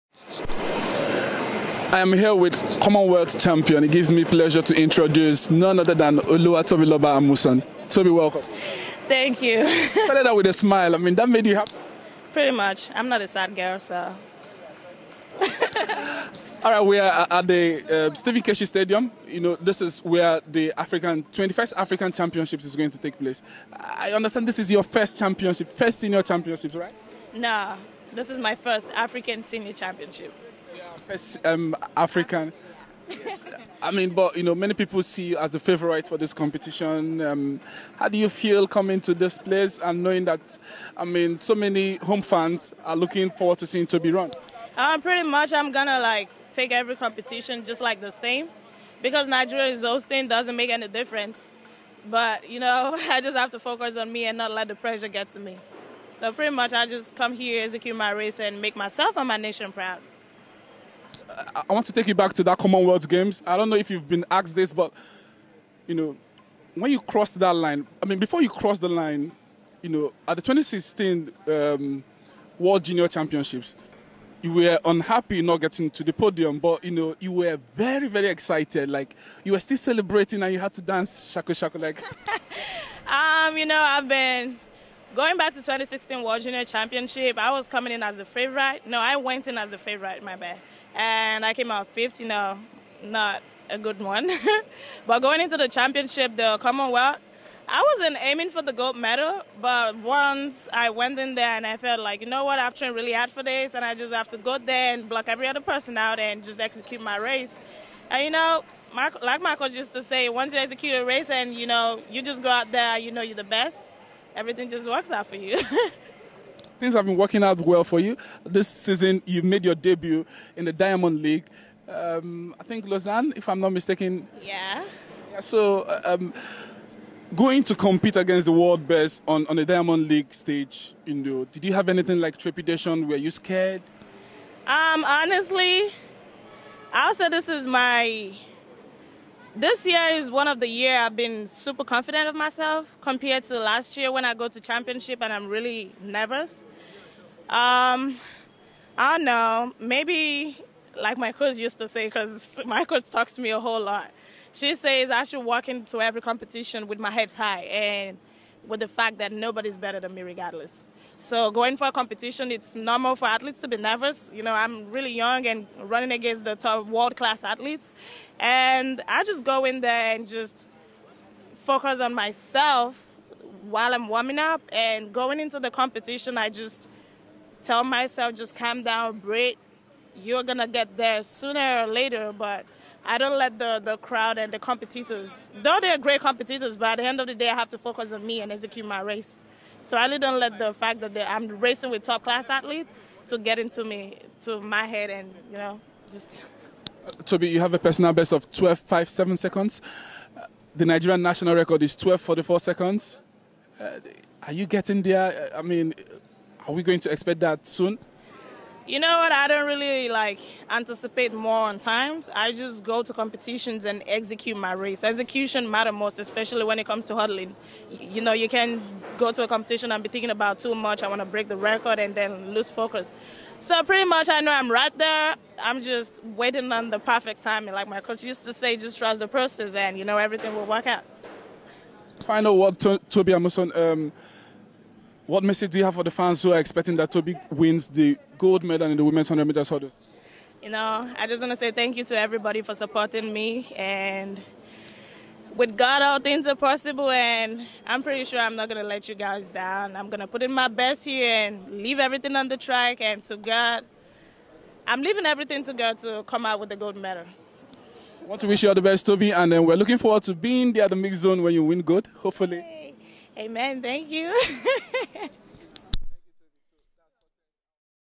Commonwealth Games champion, Tobi Amusan not fazed by the pressure of competing at home
Oluwatobiloba Amusan will be competing in her first senior African Championships, and although she is the favourite for the 100m Hurdles title, she won't let the pressure of competing at home get to her as she aims for a good outing.